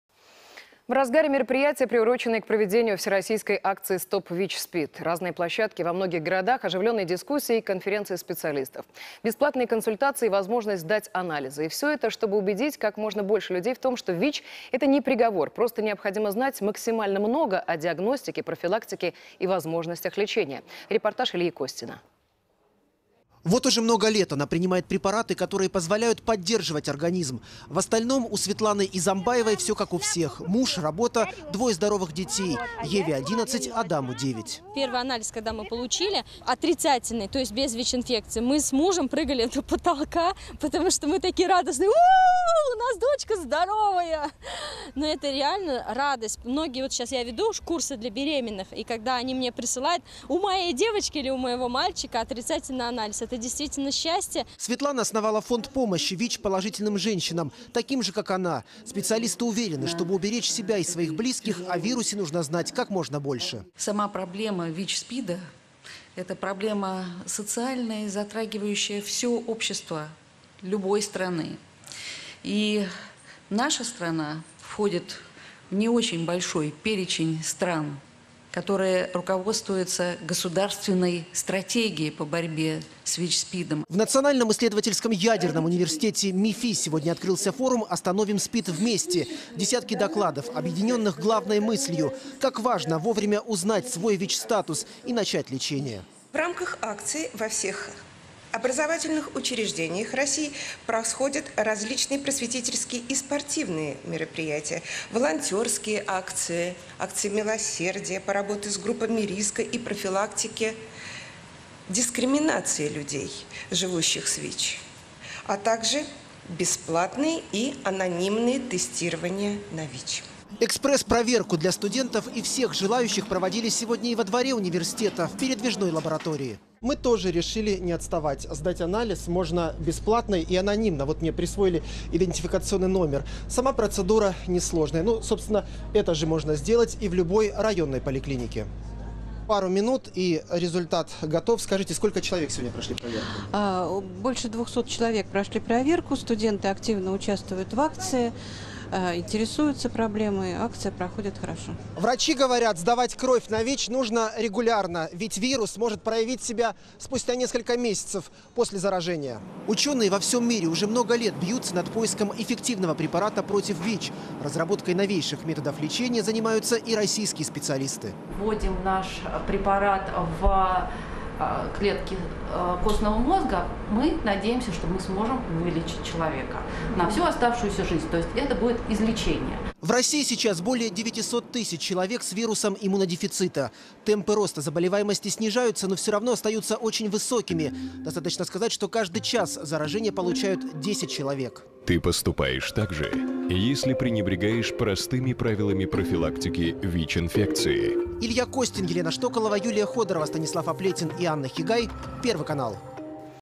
Всероссийская акция «Стоп ВИЧ/СПИД»
В рамках Всероссийской акции «Стоп ВИЧ/СПИД» в НИЯУ МИФИ прошел пятый Всероссийский открытый студенческий форум «Остановим СПИД вместе!»
Спикеры форума акцентировали внимание на мировых и российских статистических данных о ВИЧ и СПИД, а также о последних методах диагностики, профилактики и лечения.